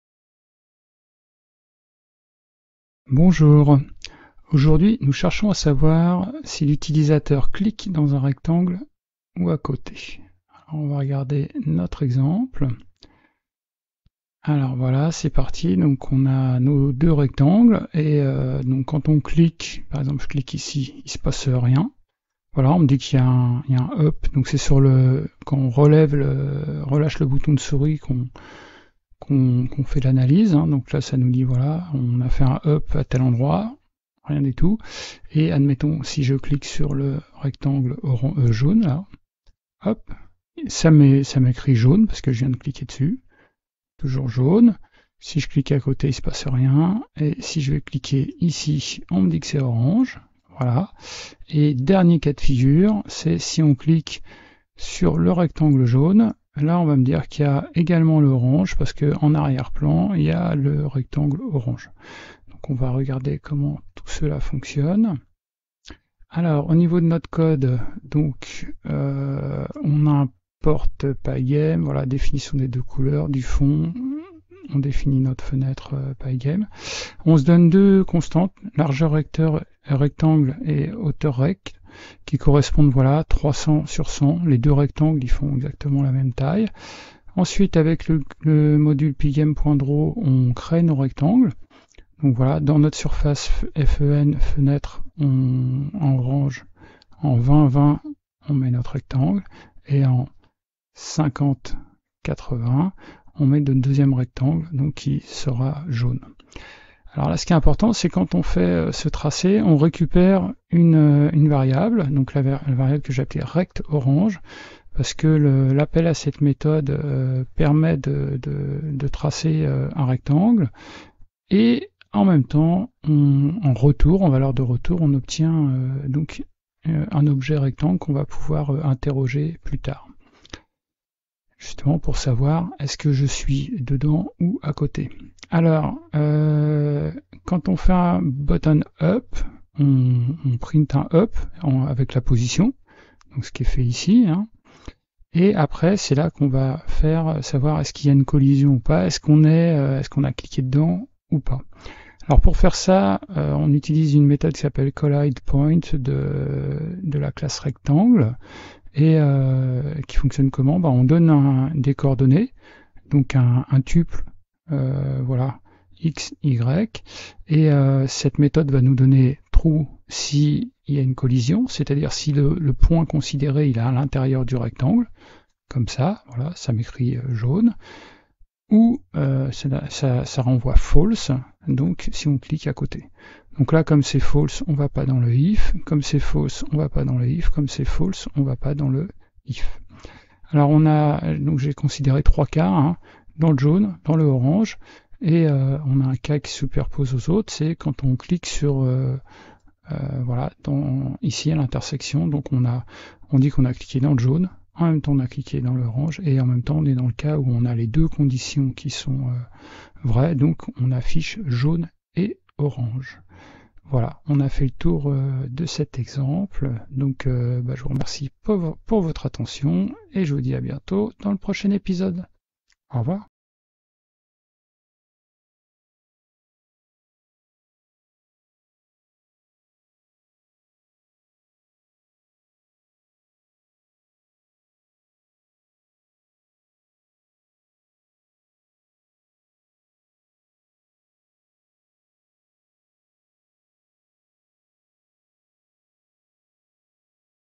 Pygame - Épisode 14 - Cliquer dans un rectangle … ou non
👉 Comment savoir si l'utilisateur a cliqué dans un rectangle de notre fenêtre Pygame ?

📋 00:00 Initiation à Pygame - Épisode 14 - Cliquer dans un rectangle … ou non
00:10 Fonctionnement de l'exemple
01:05 Analyse du code de notre exemple …
01:18 Création des deux rectangles
02:16 Savoir si l'utilisateur a cliqué dans un rectangle avec collidepoint( )
03:47 Et ensuite ?